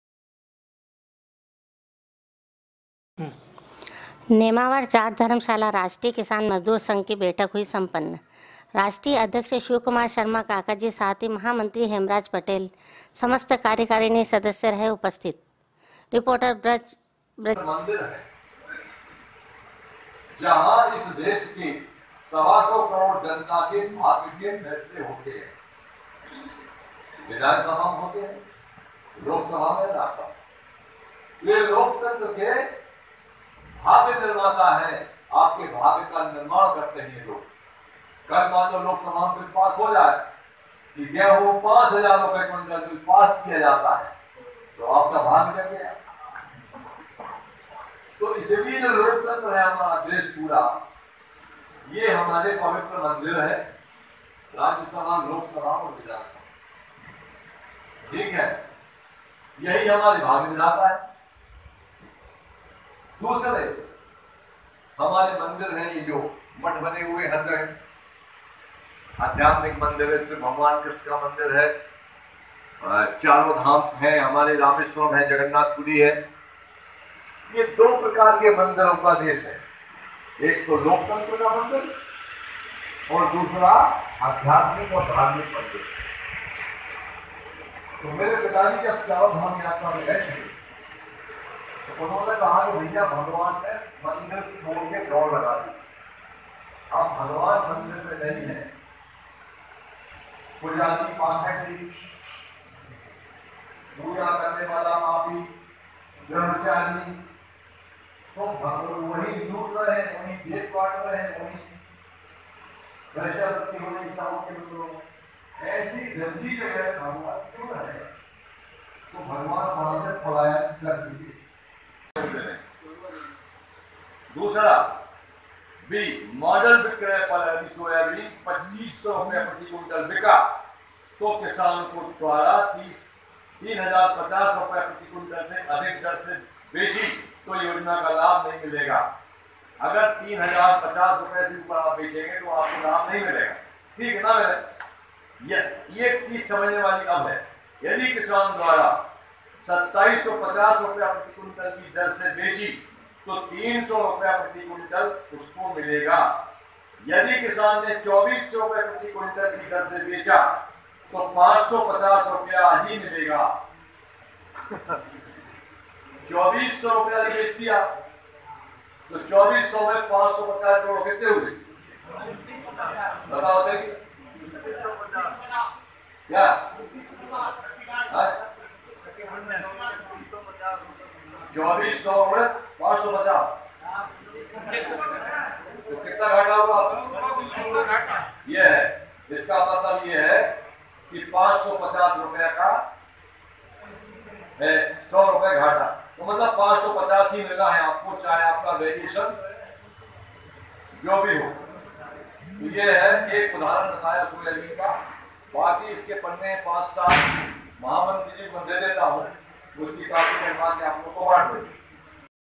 राष्ट्रिय किसान मजदूर संघ के राष्ट्रिय अध्यक्ष शिवकुमार शर्मा (काकाजी)
ने निमवार में जाट धर्मशाला में किसानो के बिच ली बैठक
संघ के महामंत्री हेमराज पटेल रहे उपस्थित
प्रदेश अध्यक्ष देव नारायण पटेल
साथ ही सभी जगह के किसान रहे मौजूद